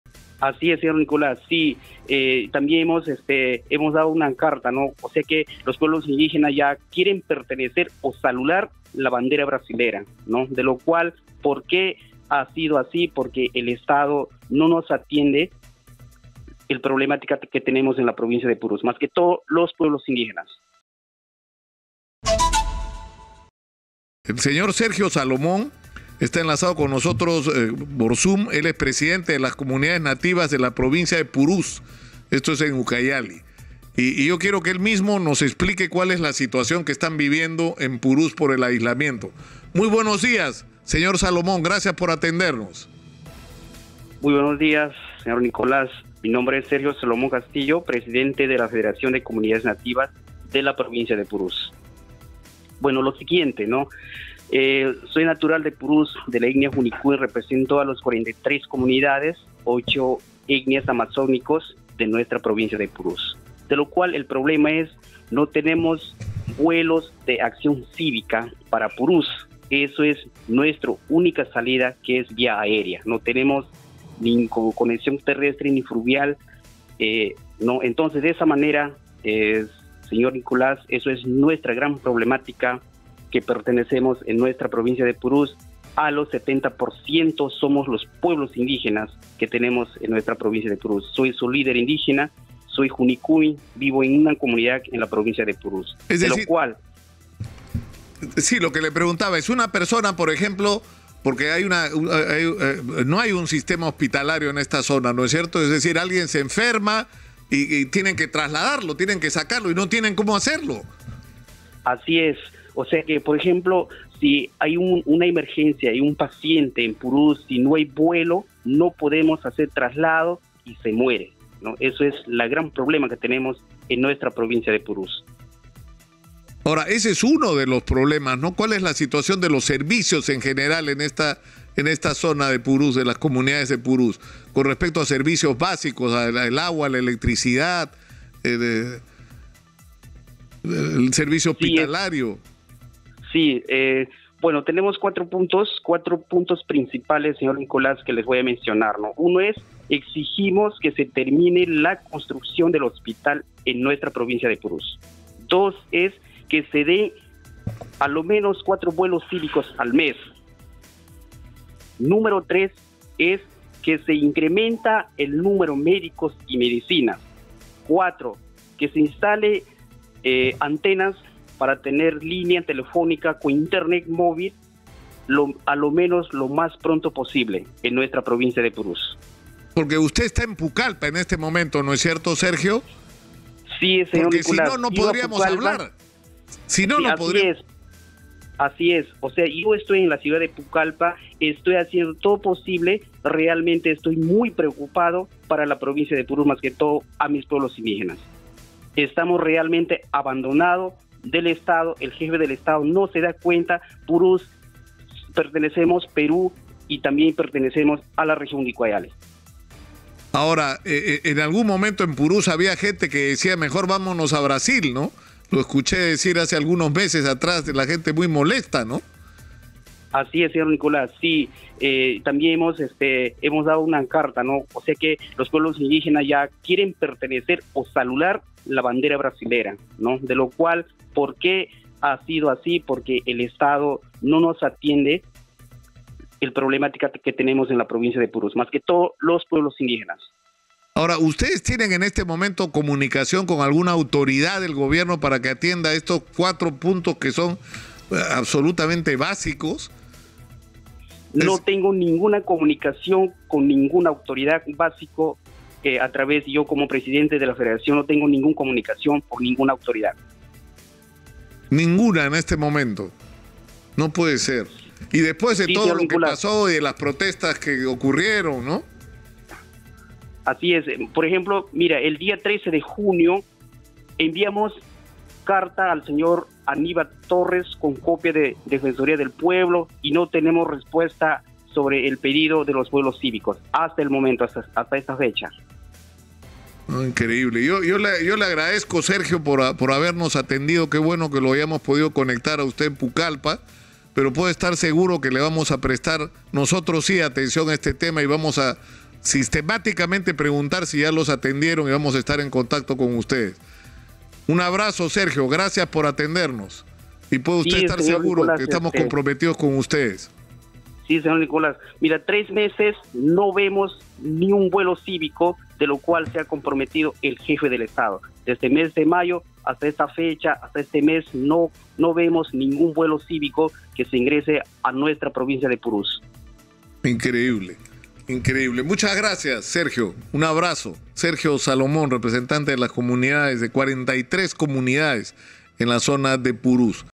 🔴🔵Ucayali: Comunidades nativas de Purús quieren pertenecer a Brasil ante olvido del Estado peruano
Este miércoles, en entrevista con Exitosa, el presidente de la Federación de las Comunidades Nativas de la provincia de Purús (Ucayali), Sergio Salomón Castillo, expresó el deseo de su sector de pertenecer a Brasil ante el olvido del Estado peruano.

Exitosa Perú
"La voz de los que no tienen voz"

Corporación Universal